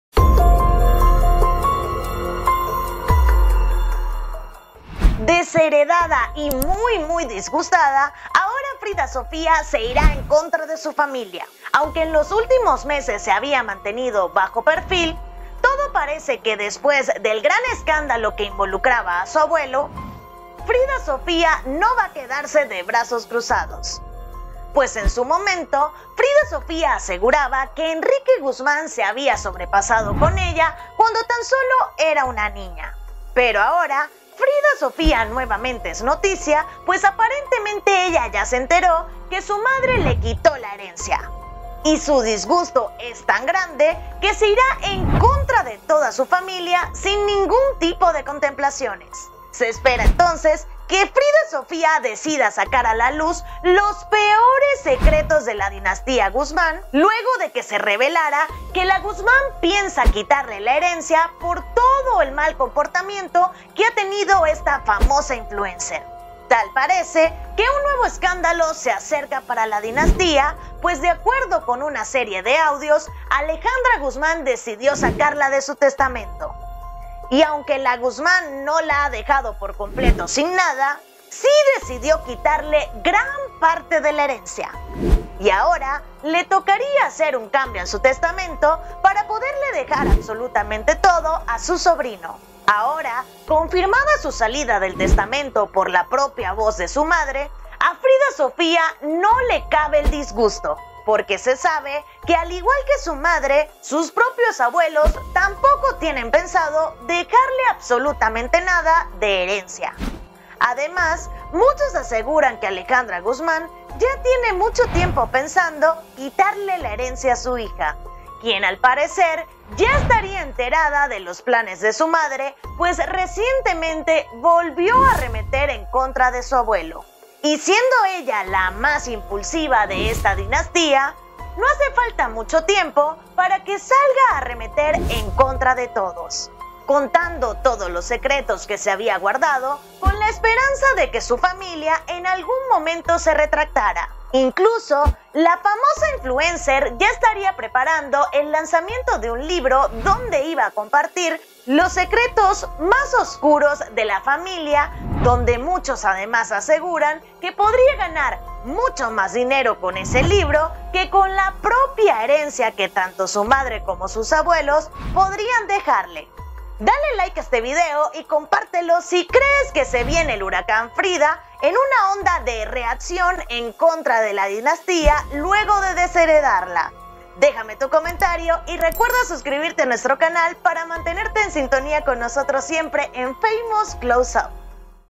🔴 Frida Sofía 𝗻𝗼 𝘀𝗲 𝗾𝘂𝗲𝗱𝗮 𝗱𝗲 𝗯𝗿𝗮𝘇𝗼𝘀 cruzados tras ser desheredada
No tendrá contemplaciones y sacará a luz nuevos detalles de su familia…